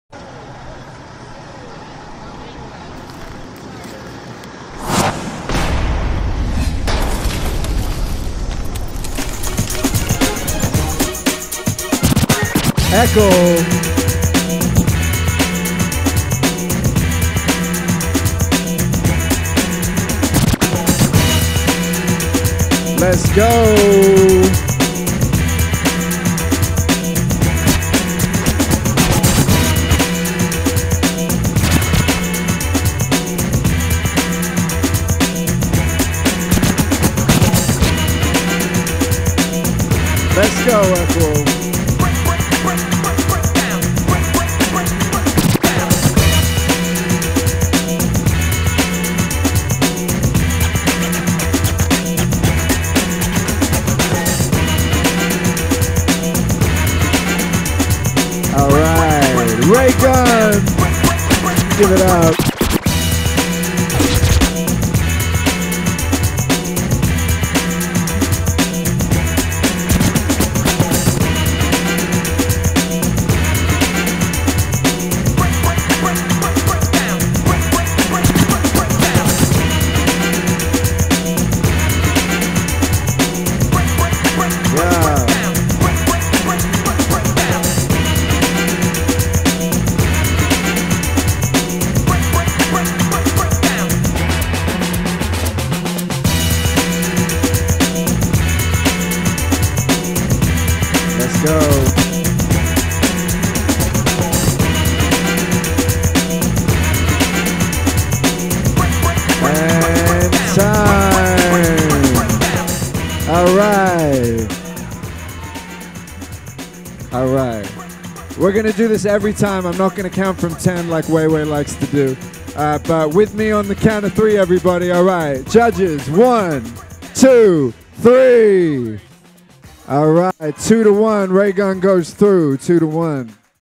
EKL vs Raygun | TOP16 | 1v1 Breaking | DESTRUCTIVE STEPS 12 LIVE
DESTRUCTIVE STEPS 12
Presented by DSDA Inc.

Supported by: City of Sydney, Redbull AU, Monkey Baa Theatre.

Judges:
Poe One (Style Elements)
Kamikazi (DMC/Juse Crew)
Hideboo (RAW/Common Ground)

MC: Chux Montano (Riddim Nation)
DJ: Adverse (The Groove Dealers)

Intro by Michael Clark
Filmed by @opnsrc.co
Photography by @photography.jny

DESTRUCTIVE STEPS STREET DANCE FESTIVAL